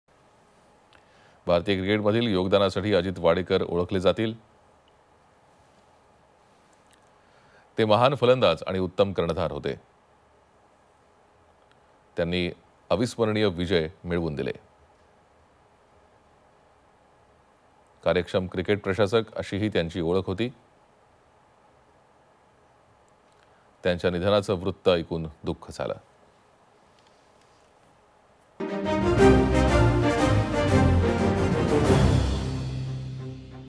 नवी दिल्ली| अजित वाडेकर महान क्रिकेटपटू- मोदी
Twitter | PM Modi Pay Homeage To India`s Former Captain | Ajit Wadekar